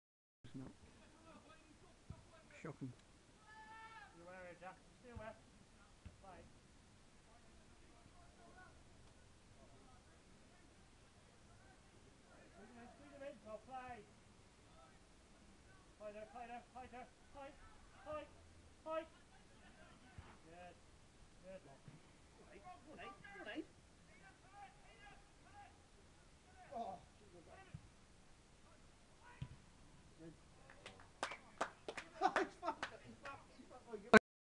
E.C.P..L Covid 19 Cup Game ,St Stephen 5 - 1 Veryan
Goal no 5 ,Aiden Le Masurier crosses for Craig Coad to fire home.